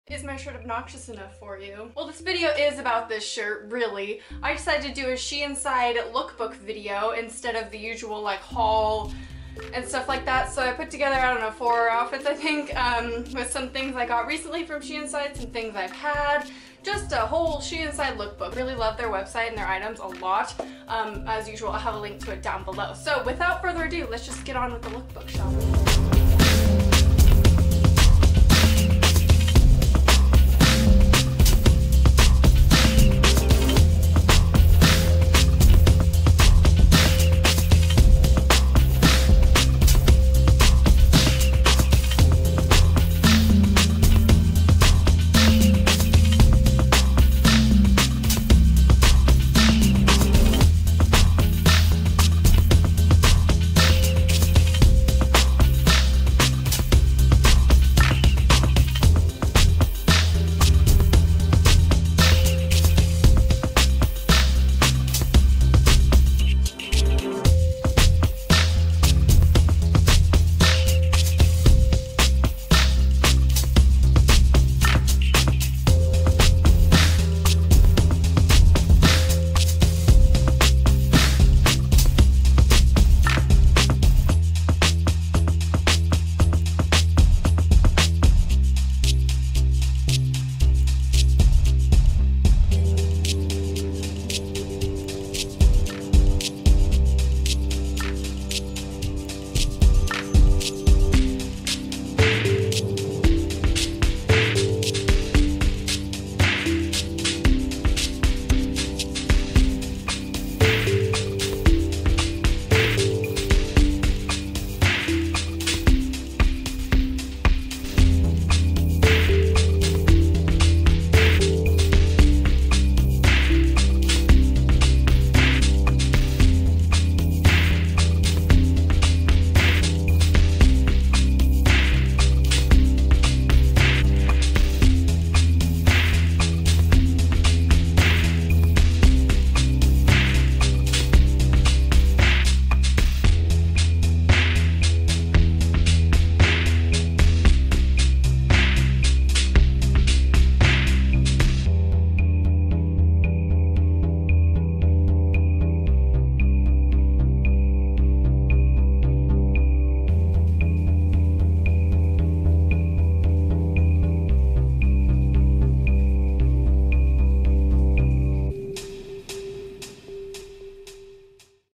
Sheinside Lookbook!
I thought I would put together a little lookbook featuring some pieces from Sheinside that I've gotten recently! Make sure to check out their website below!

Carpe Noctem Sweatshirt (sold out)

Horror Blog!